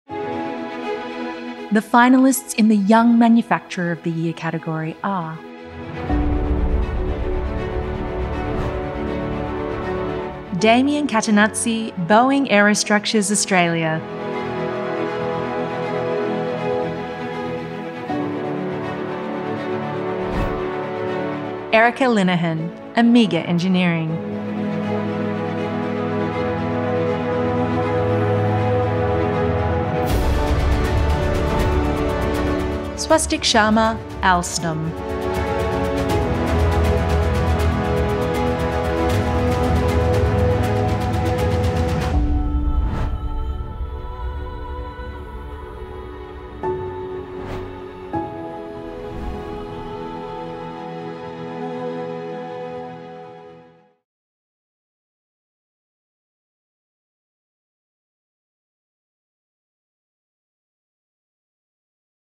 VMHOF Awards 2023 – Finalists in the Young Manufacturer of the Year category
Finalists in the 2023 Victorian Manufacturing Hall of Fame Award for Young Manufacturer of the Year category were: Swastik Sharma (Alstom),  Erica Linehan  (Amiga Engineering) and Damian Cattenazzi (Boeing Aerostructures Australia).